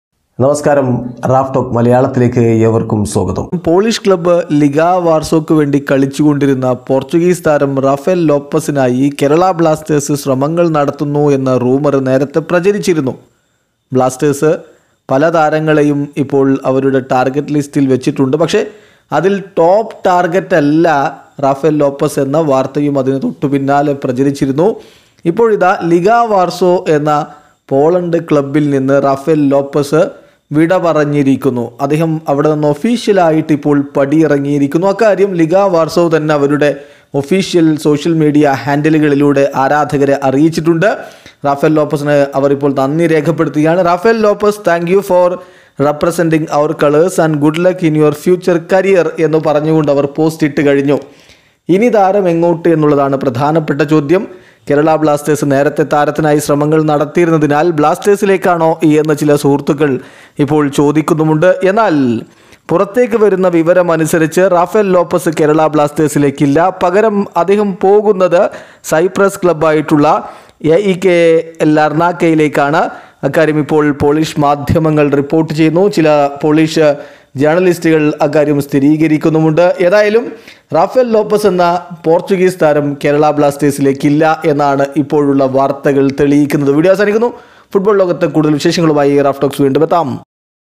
Official: റാഫ ലോപെസ് പോളിഷ് ക്ലബ്ബ് വിട്ടു, ഇനി എങ്ങോട്ട്? | KBFC News
Rafael Lopes
KBFC News